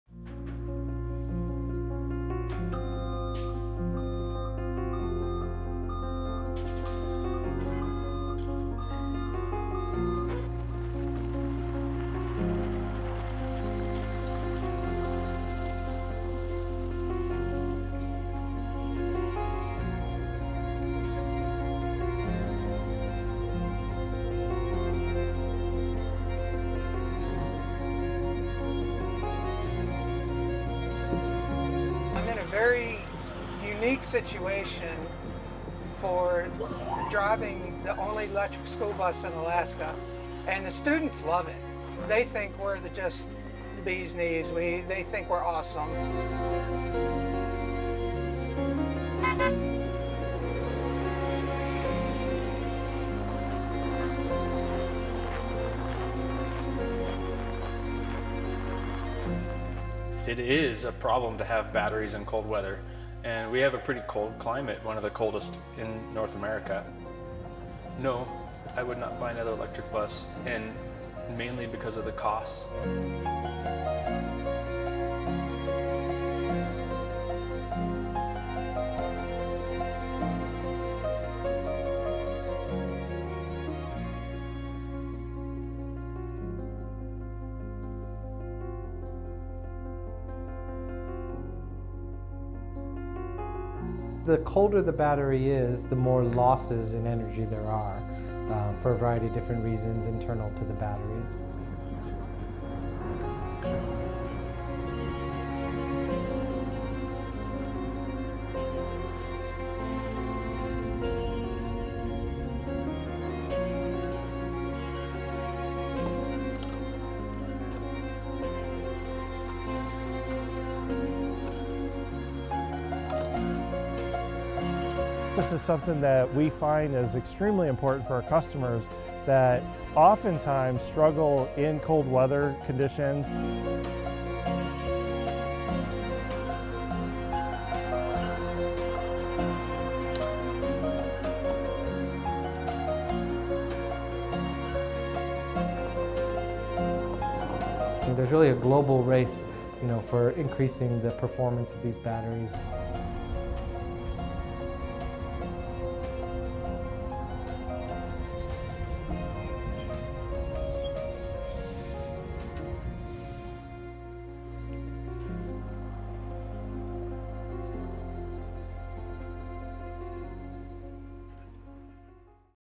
Global race is on to improve EV range in the cold
(4 Mar 2023) Many electric vehicle batteries lose power when it's very cold. Thousands are confronting the issue if they own an electric car and have to make a longer trip when temperatures dip (March 4)(AP video: Mike Householder, Mark Thiessen)